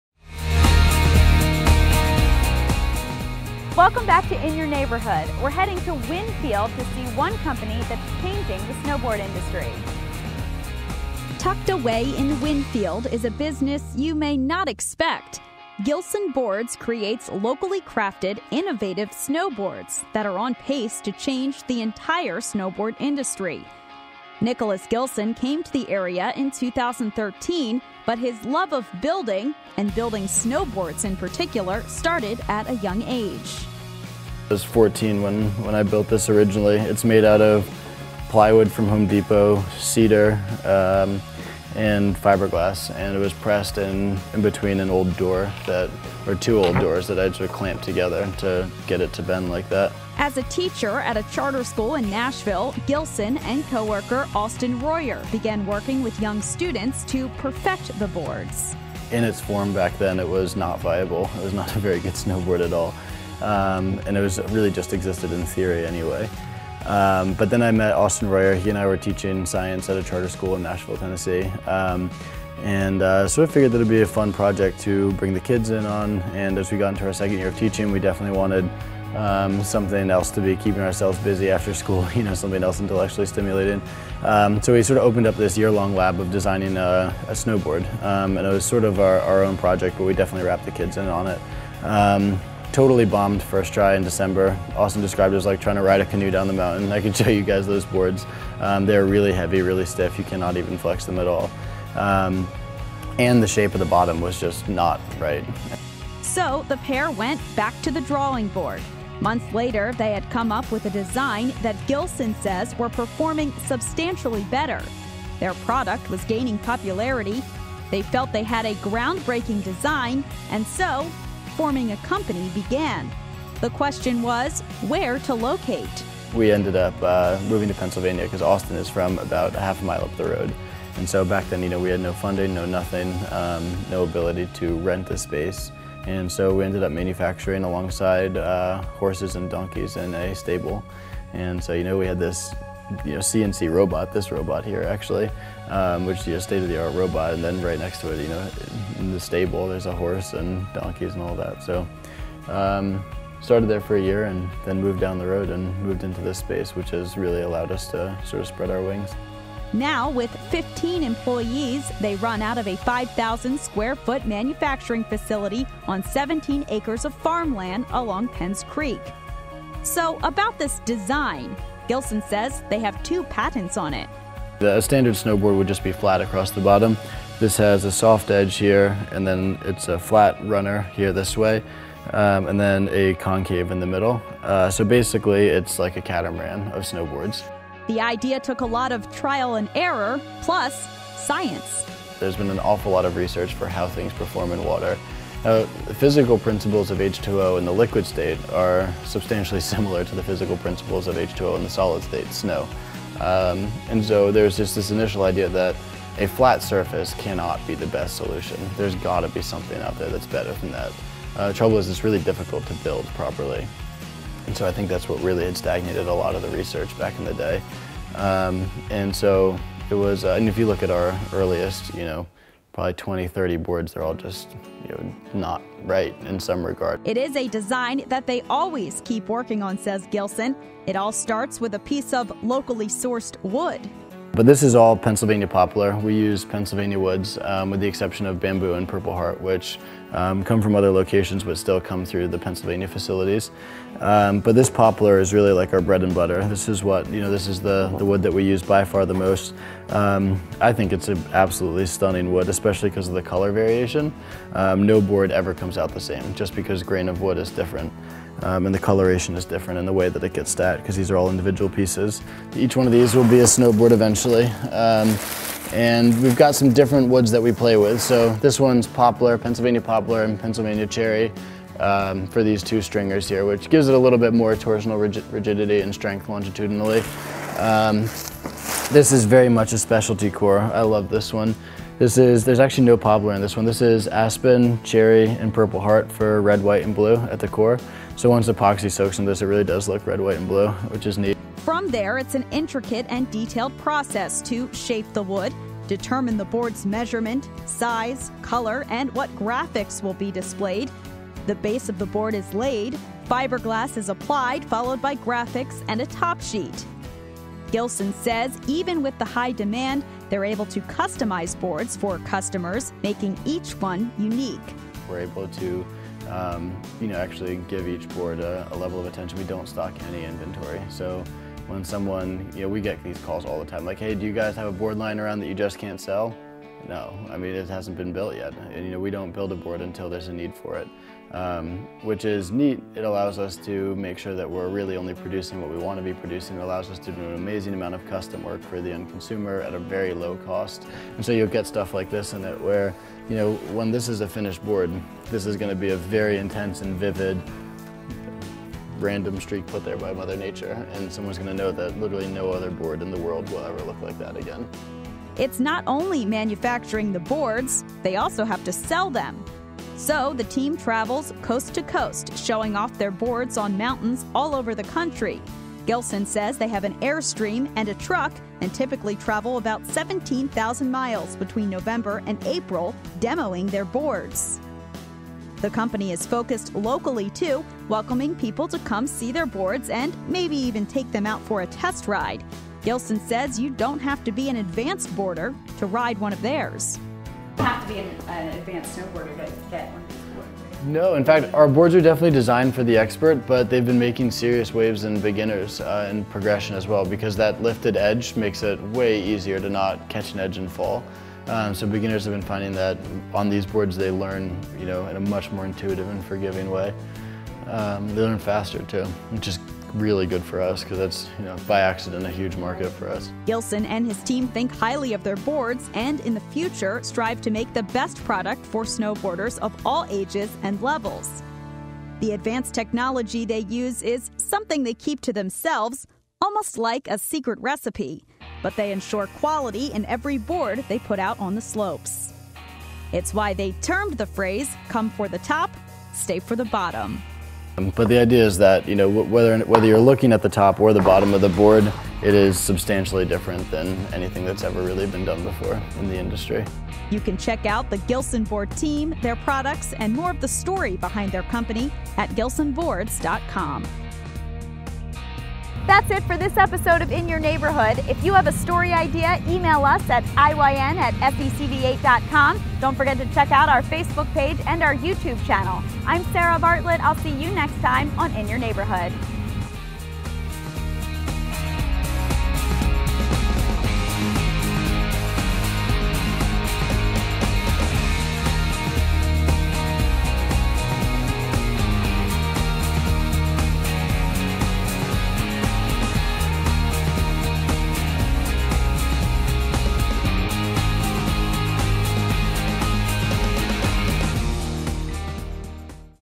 Gilson Boards
We head to Winfield to check out Gilson Boards, a snowboarding company that's on pace to change the industry.